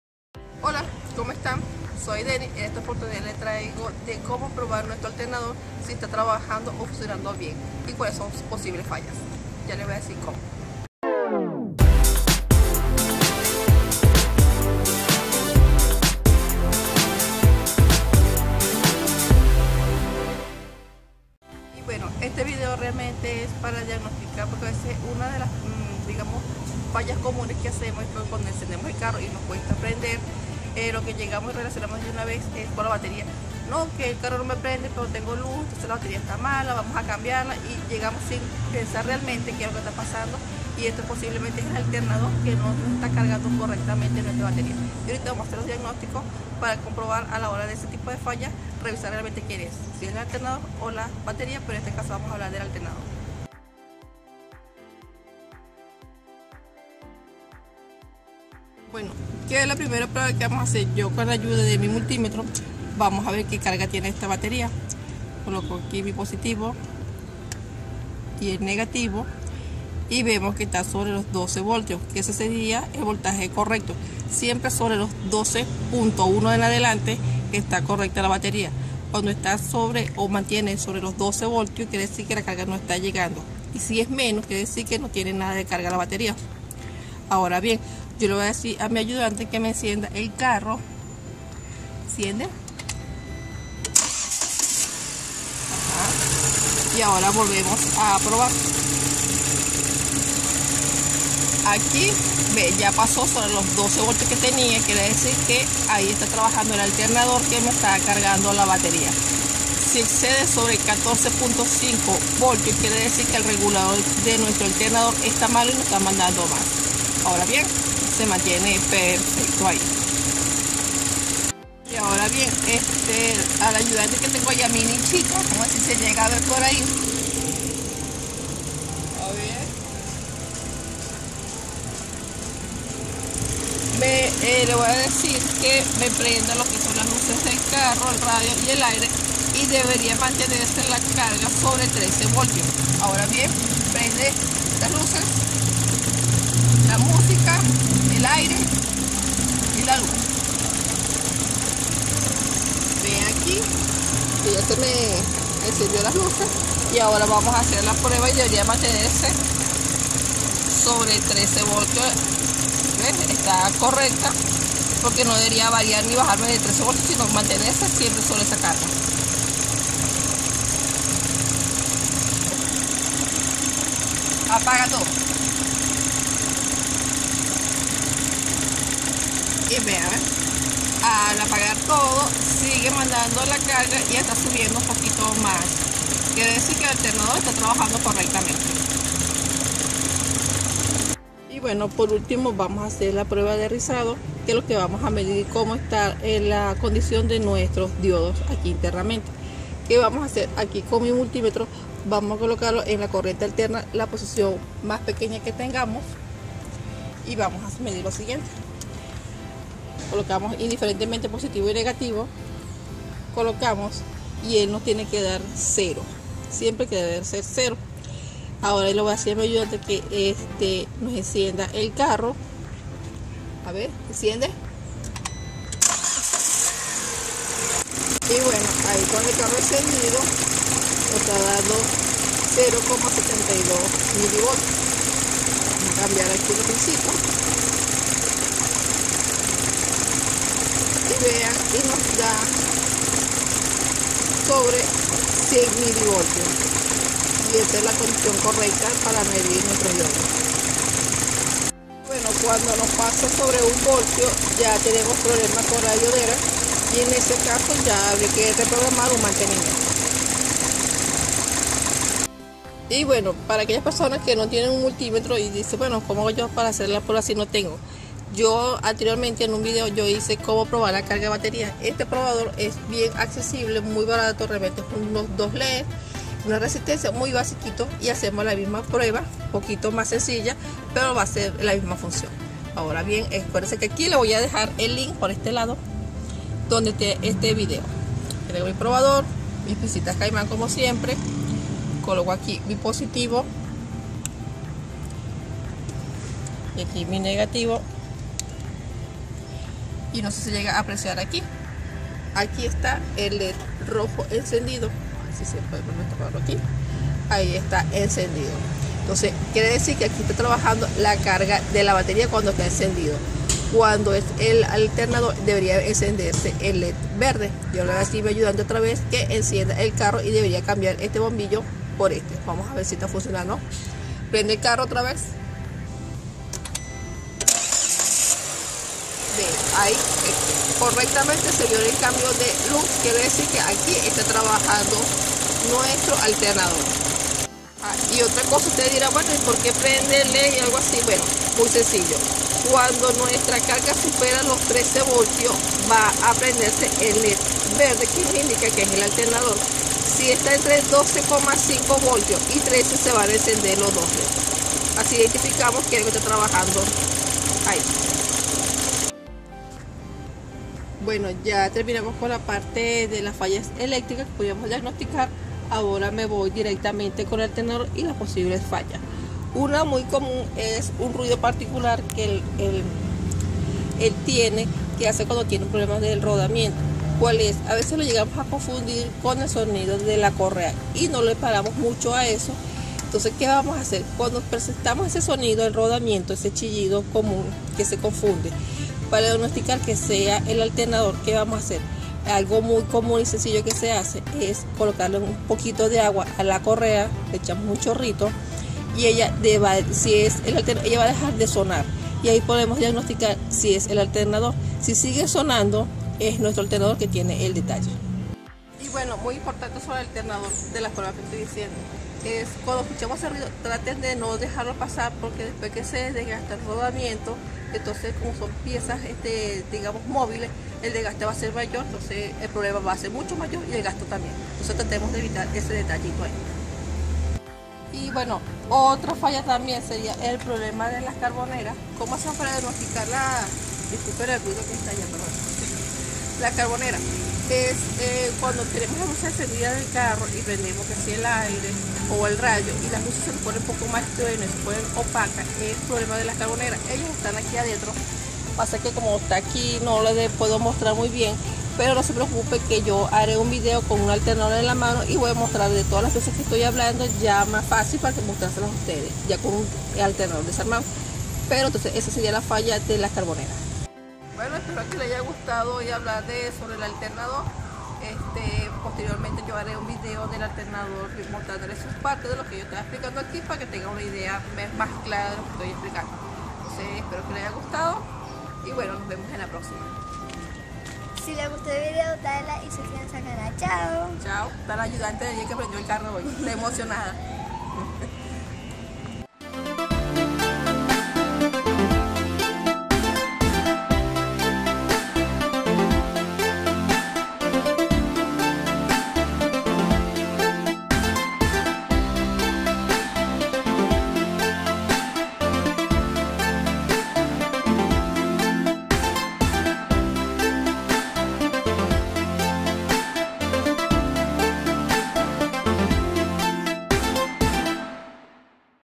✅ Como probar el alternador⚡️ del vehículo - Pruebas CON y SIN Voltímetro✔️
En este video muestro como realizar algunas pruebas que te ayudaran a comprobar si tu alternador esta cargando la batería. Tambien incluyo un probador para los que no tienen el multímetro.
Como siempre, hazlotumismo y practica la mecánicaautomotriz.

Al ver este video podrás:
🔷 Realizar pruebas a tu alternador.
🔷 Saber cuales son los voltajes de carga del alternador.
🔷 Conocer las fallas eléctricas y mecánicas del alternador.

➕ ¿Saber más de lo que hice hoy?

¿Preguntas y/o sugerencias?
🖥️ A traves de mi blog en intarcesoft/es/automotriz